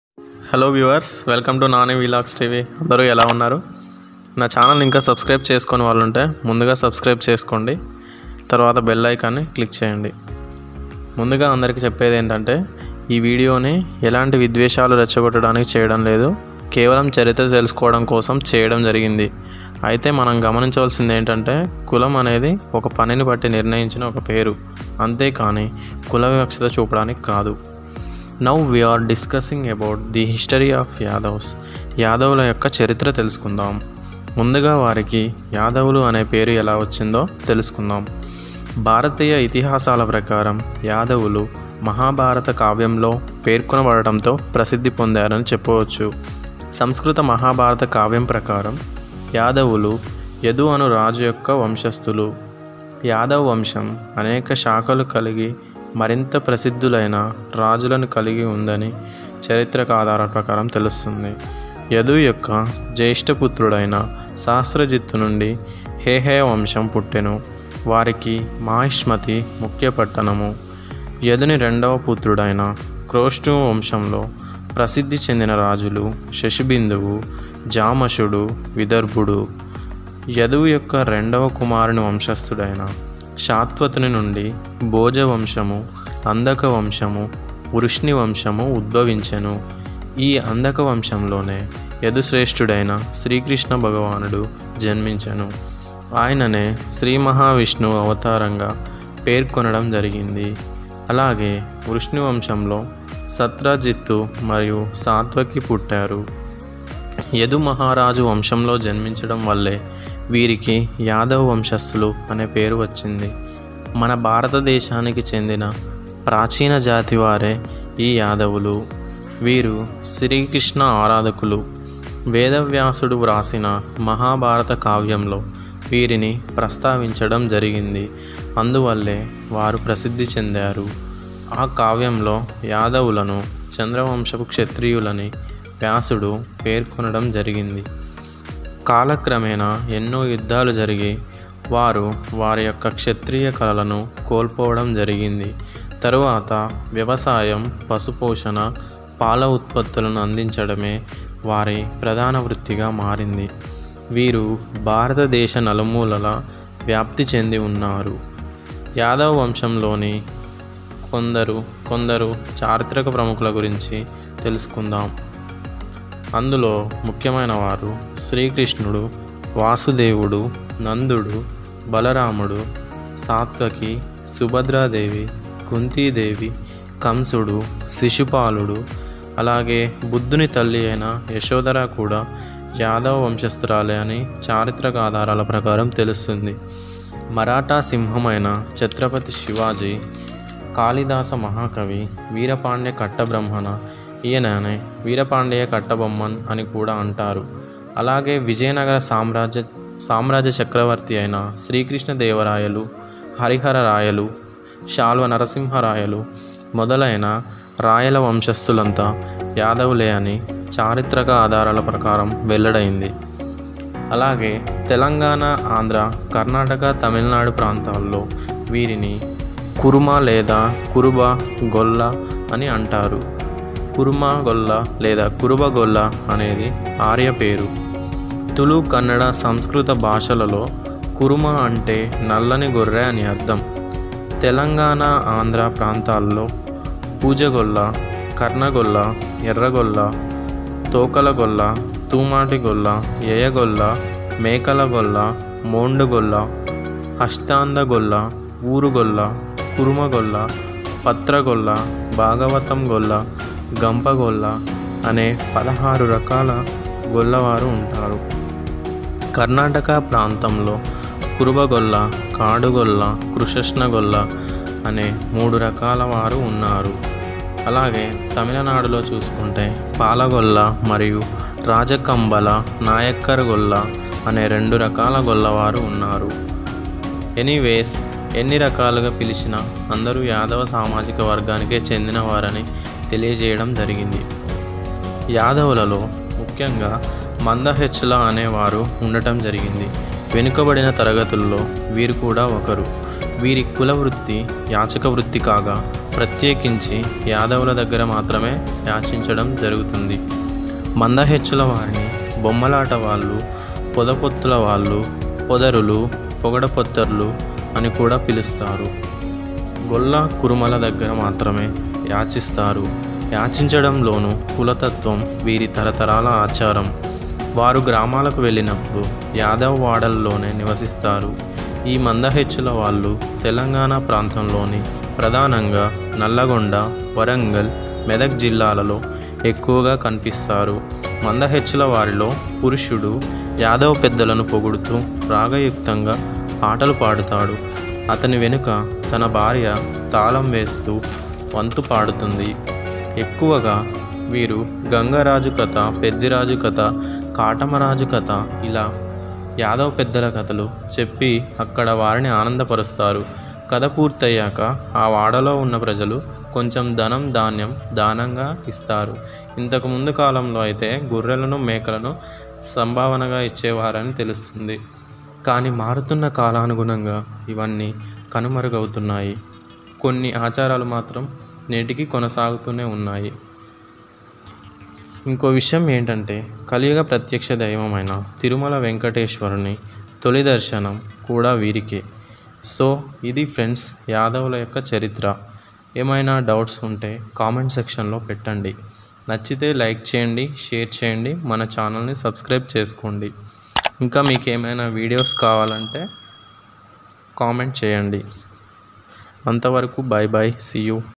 yadav's history in telugu//by nani vlogs tv// lord sri krishna's history in telugu//nani vlogs Tv/
yadavs must watch this video,this is only the video about the history of indian yadav' kings.